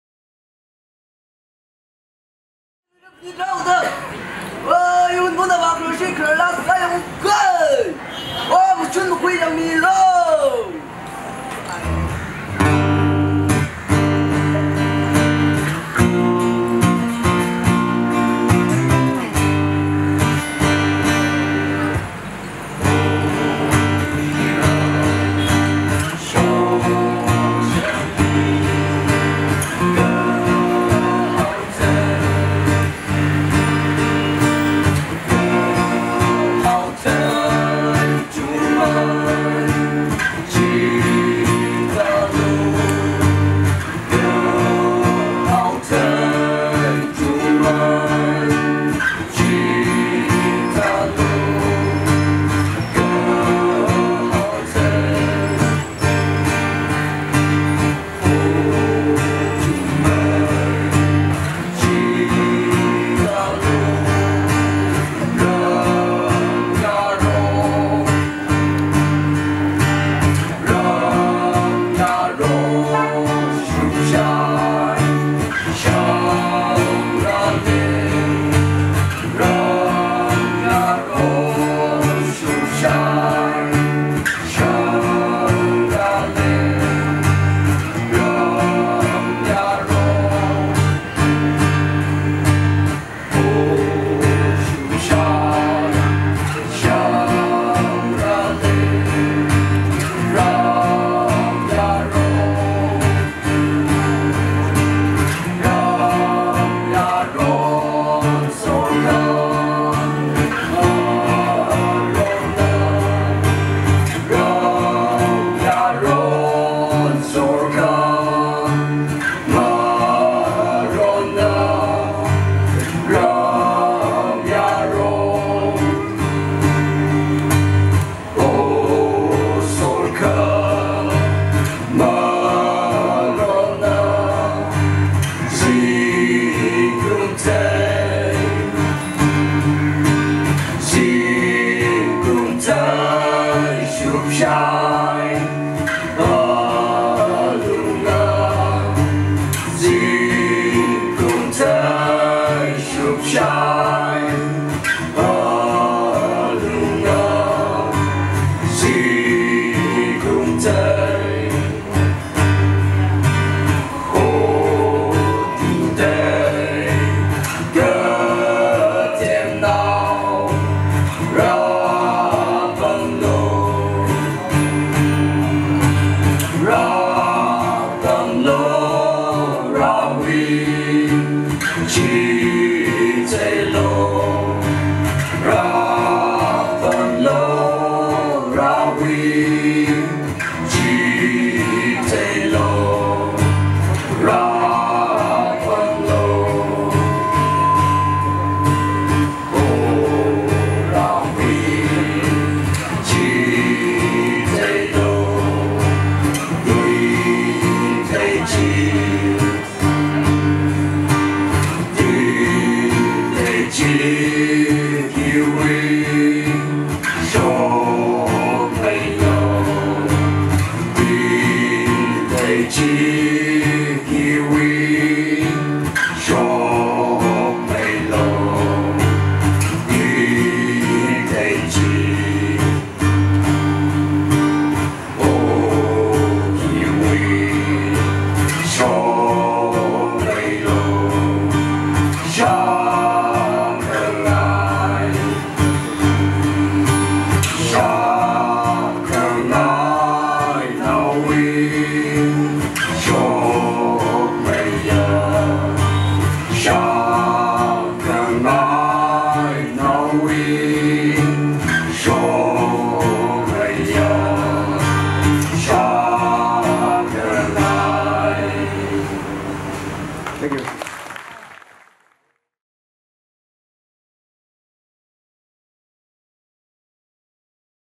I Hunphun Goa
Tangkhul Welfare Union Goa
1st Luira Phanit folk Song Competition
(1st Runner's up) 2019.